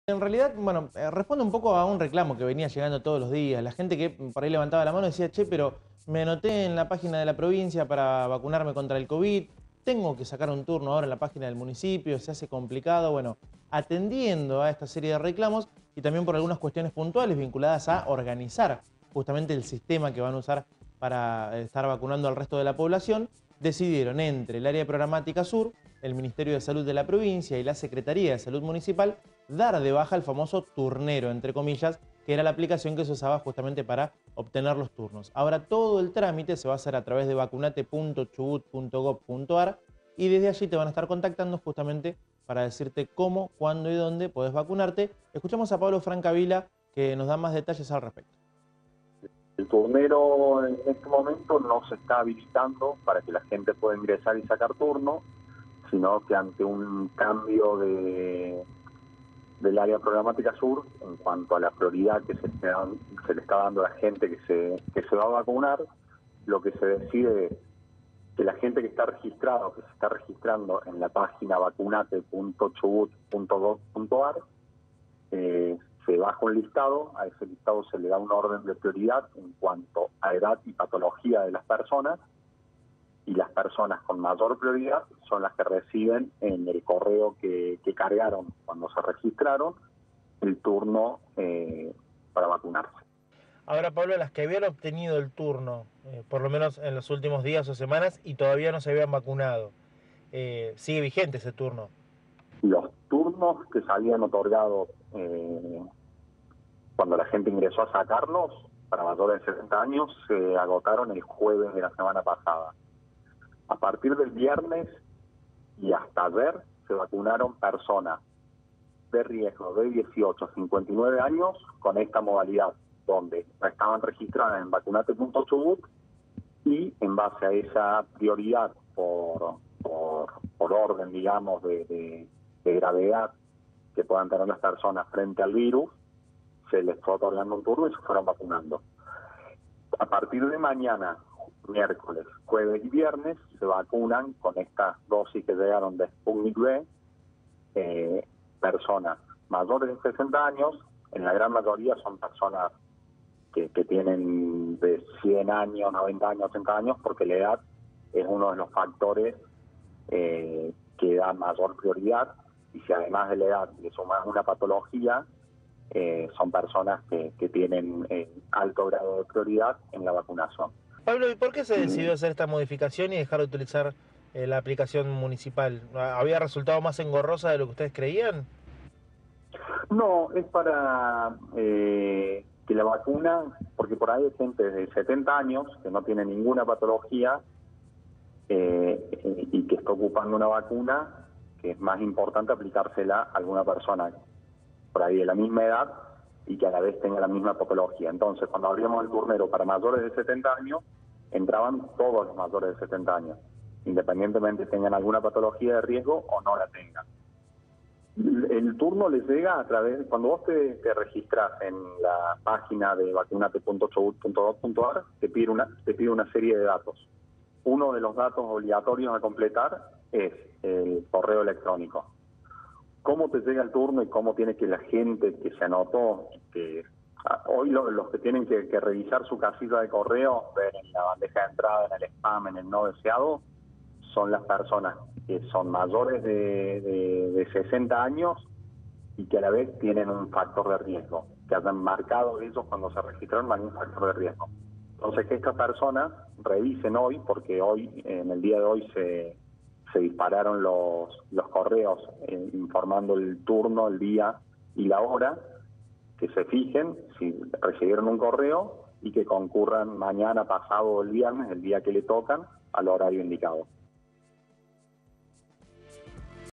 Queda sin efecto la web para sacar turnos del municipio
Esto lo decidieron en conjunto con autoridades del area programatica sur, debido a que directamente desde la web de provincia (vacunate.chubut.gov.ar) se ordenara a los anotados con un orden de prioridades dependiendo de la edad y las patologias que tenga cada uno, y directamente se los convocara por mail. A partir de ahora entonces, sera una sola la via de acceso a la vacunacion.